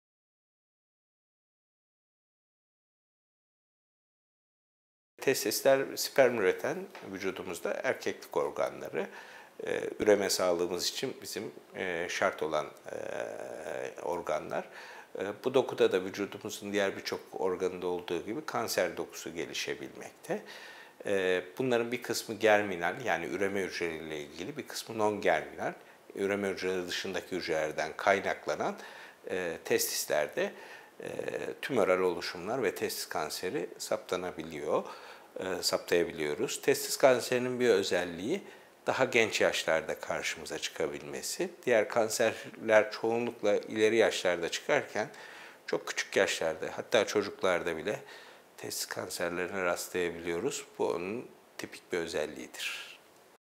Testis kanseri nedir?
AcıbademKadıköy Hastanesi, Üroloji Uzmanı, Doç. Dr. Fehmi Narter; "Testis Kanseri" üzerine soruları cevaplıyor.